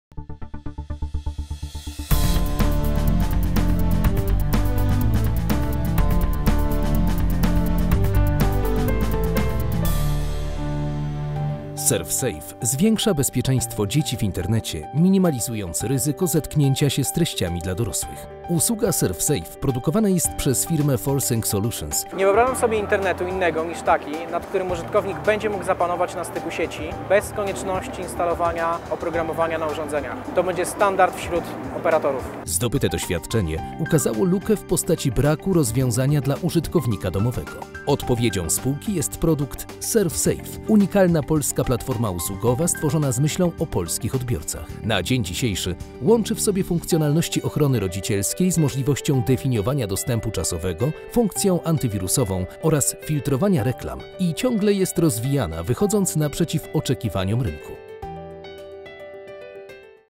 Surfsafe - unikalna polska platforma usługowa
Surfsafe  zwiększa bezpieczeństwo dzieci w Internecie minimalizując ryzyko zetknięcia się z treściami dla dorosłych.